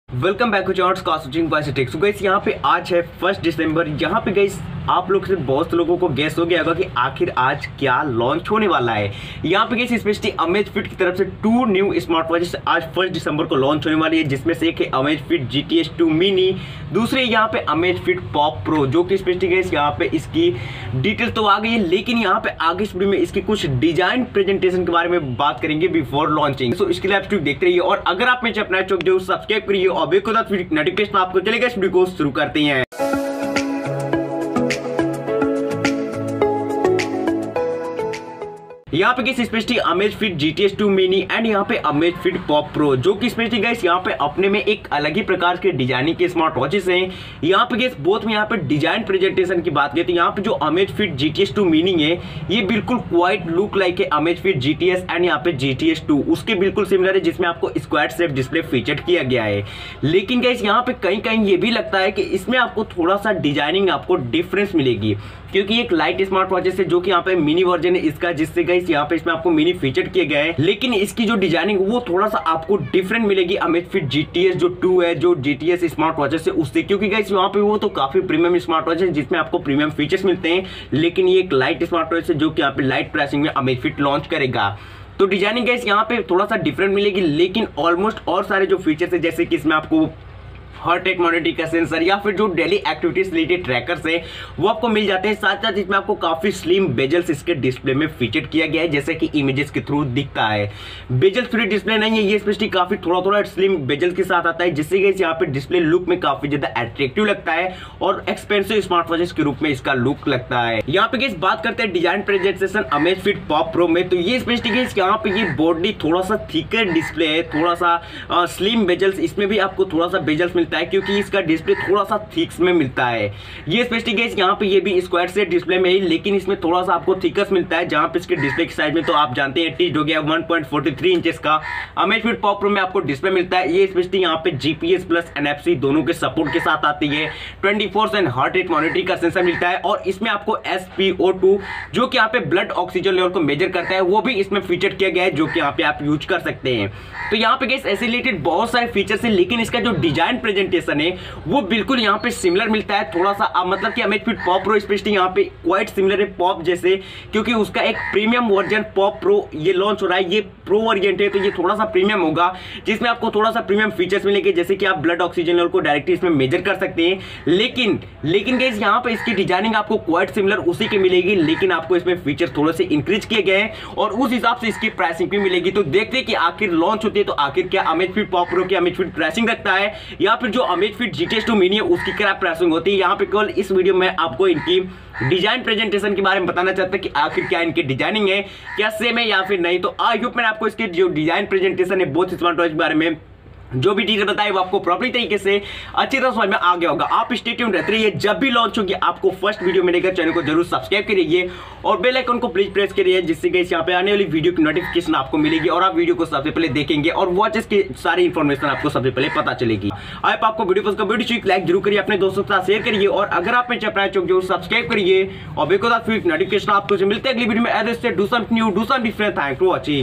Amazfit GTS 2 Mini & Amazfit Pop Pro | Launching Today on 1 December | Amazfit New Smartwatch
Hai Guys Es Video Me Hm Apko Batayenge Ki Amazfit Two New Smartwatch Aj Launch Ho Rhi Hai 🔥🔥🔥